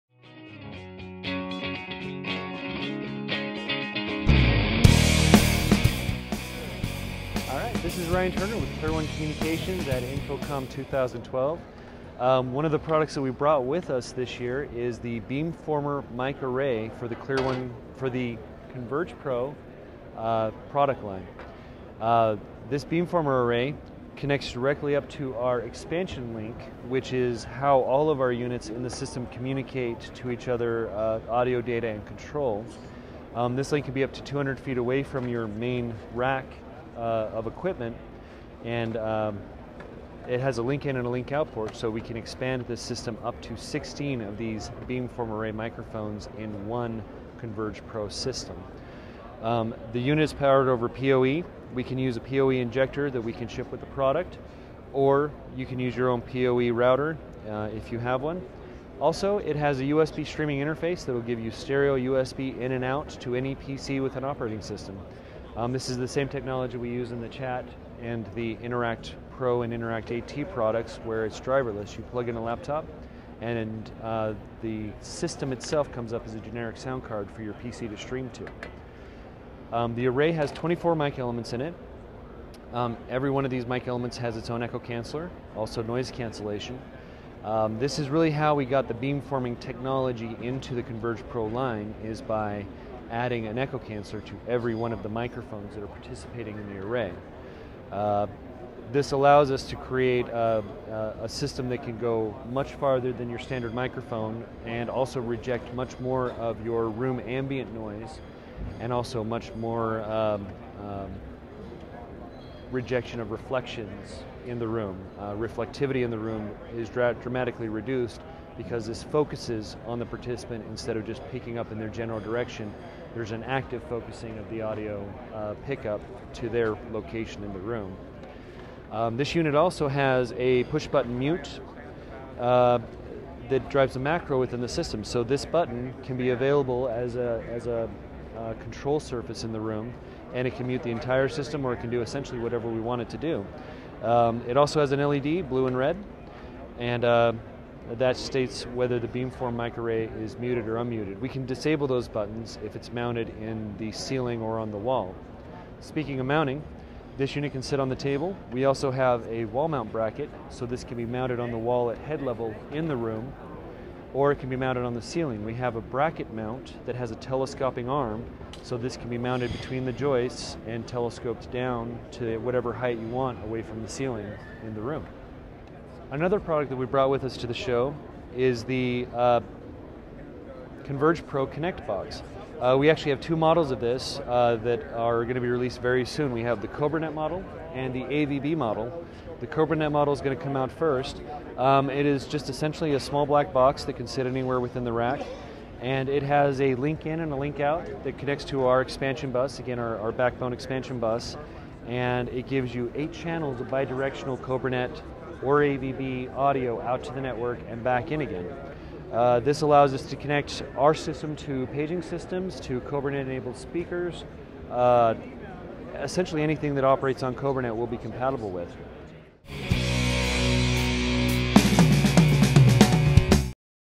NewComm Technologies presents The ClearOne Beamforming Microphone Array at INFOCOMM 2012
ClearOne Beamforming Microphone Array -- Ryan Turner provides an overview of the revolutionary Beamforming Microphone Array.  Sure to forever change the way we design a conferencing system, this mic will amaze you with its capabilities.